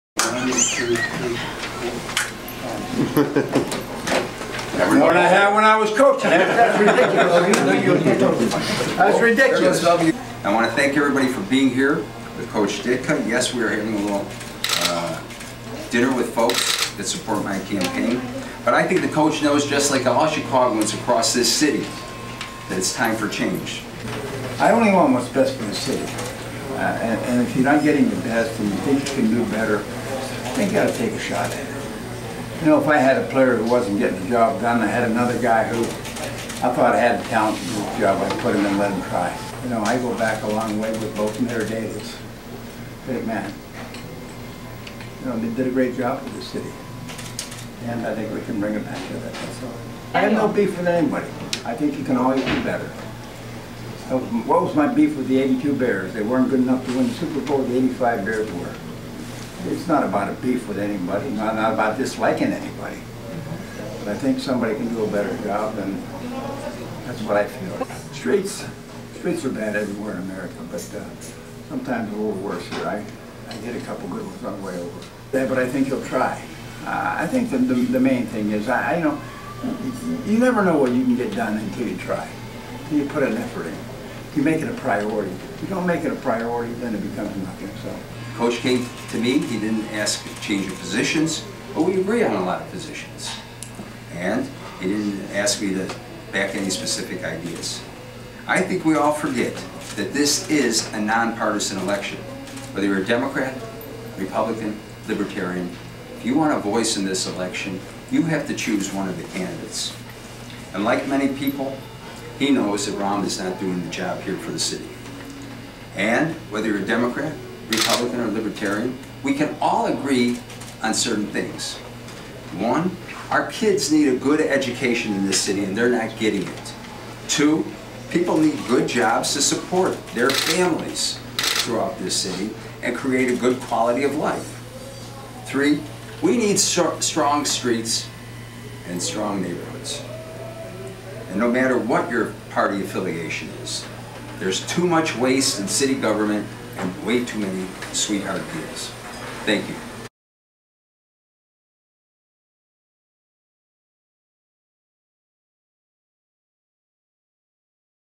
Ditka fudraiser for Fioretti
Former Bears coach Mike Ditka (left) and mayoral candidate Bob Fioretti address the media Tuesday evening prior to a private fundraiser, at Ditka's restaurant, the coach is having for the mayoral campaign. | Michael Schmidt/Sun-Times